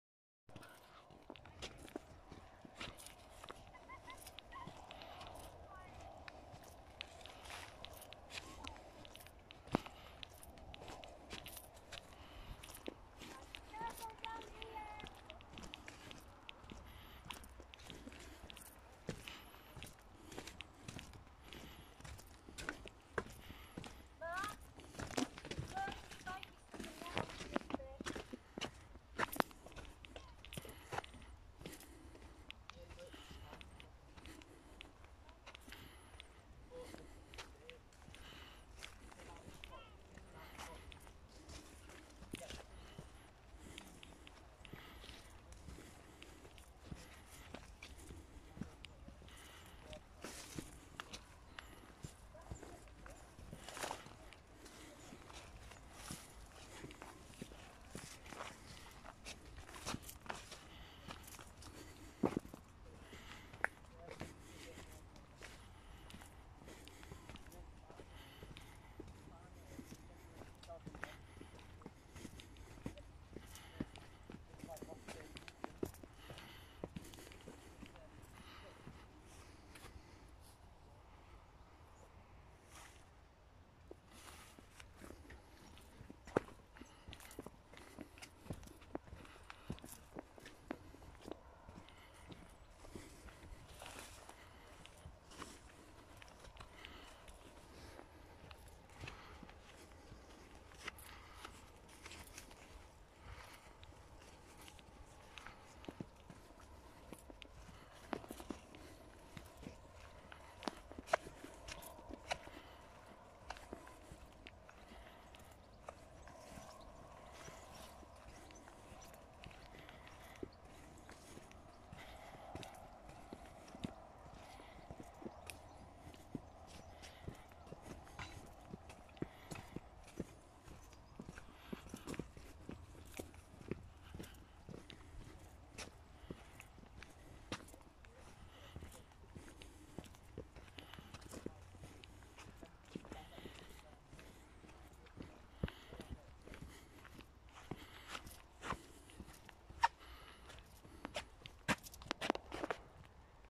Scaling Reservoir. North Yorkshire Moors.
We tackled the walk around Scaling Reservoir which is located approx  10 miles east of Whitby on the A171.
Plenty of parking (£1.50 for two hours).
The walk is around 5km and you'll need walking boots or shoes.